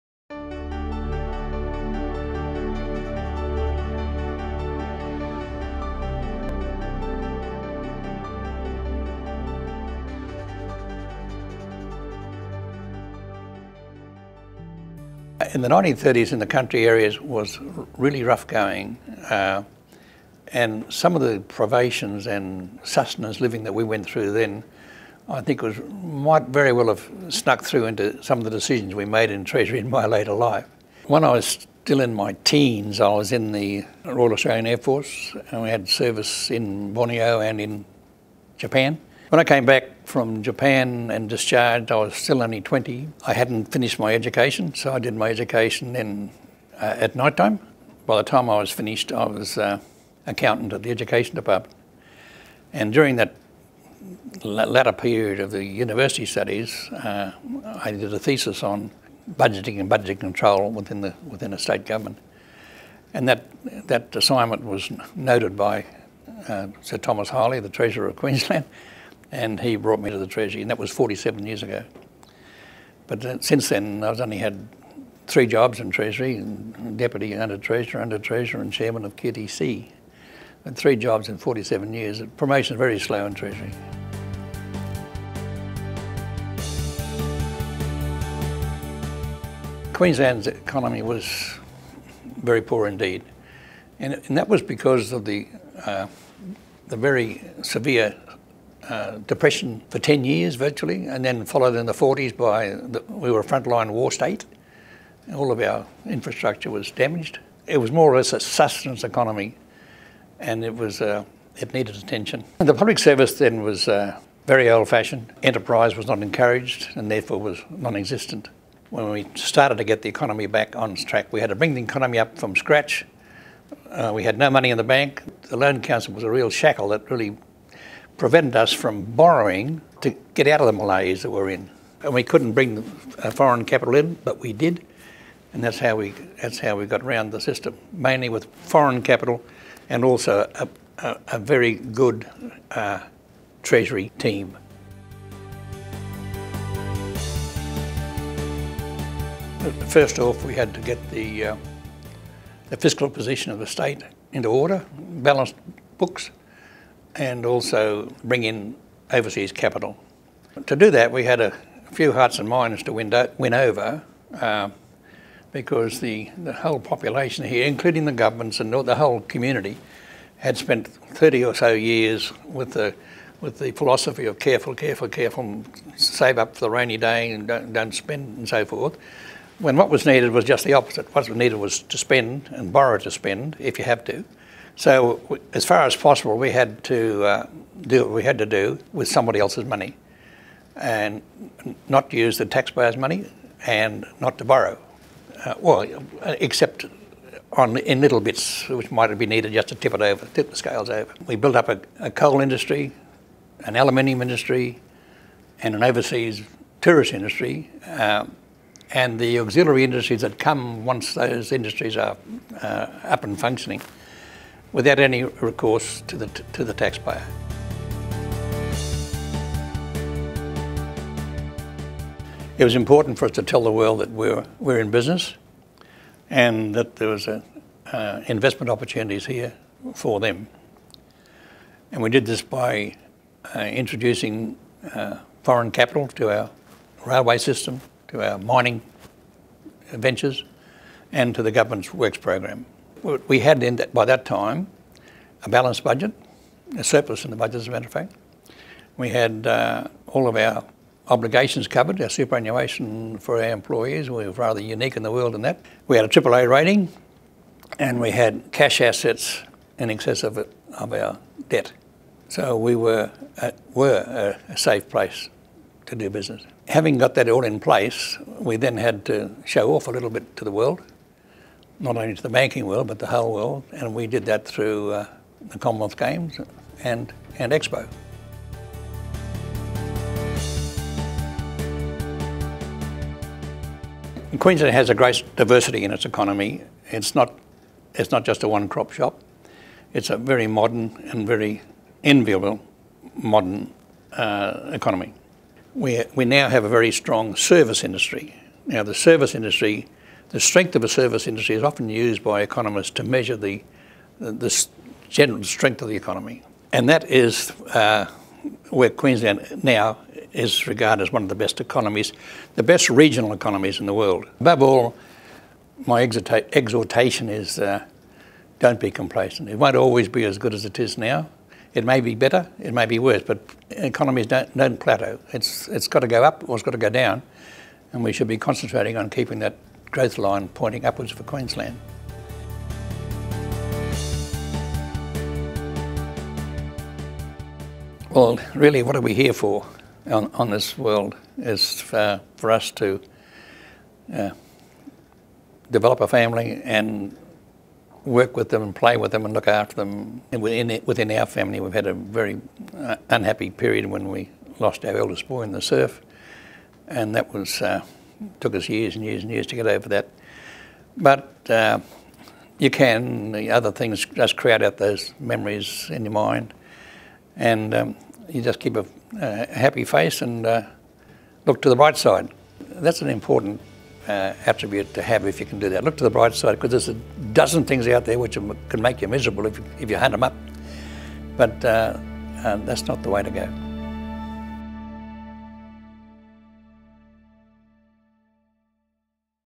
Rowland 5 Minute Focus with Sir Leo Hielscher AC
Hear from Sir Leo Hielscher AC, founding Chairman Queensland Treasury Corporation.

Visit Rowland.com.au communication, digital and creative agency for more news and information